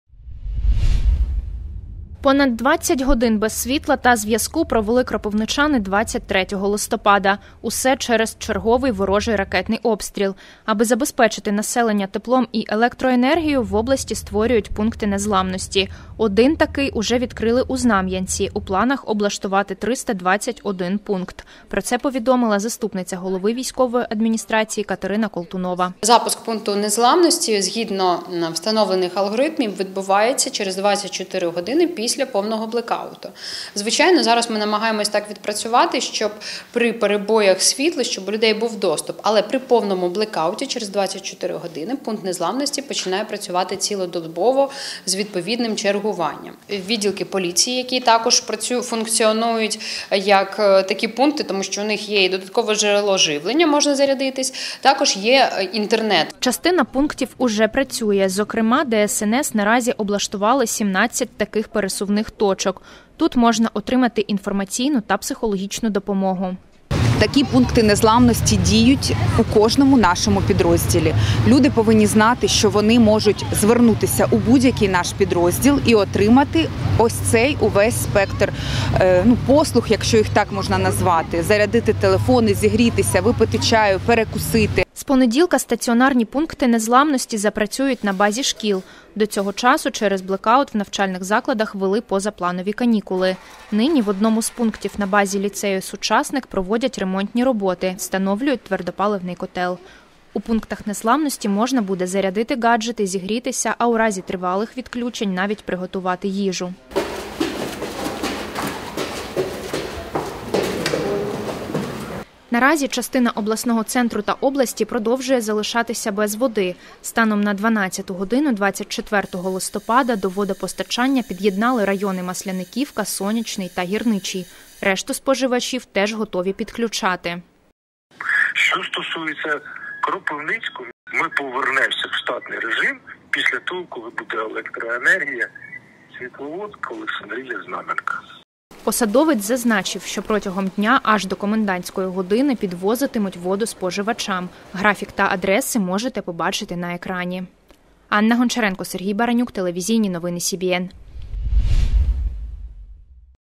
Блекаут у Кропивницькому: де зігрітись та дістати воду? | телеканал Вітер
Понад 20 годин без електроенергії та зв'язку кропивничани провели у результаті ворожих обстрілів 23 листопада. Більша частина обласного центру продовжує залишатись без водопостачання. Аби забезпечити населення усім необхідним у разі блекауту, в області розгортають 321 пункт незламності.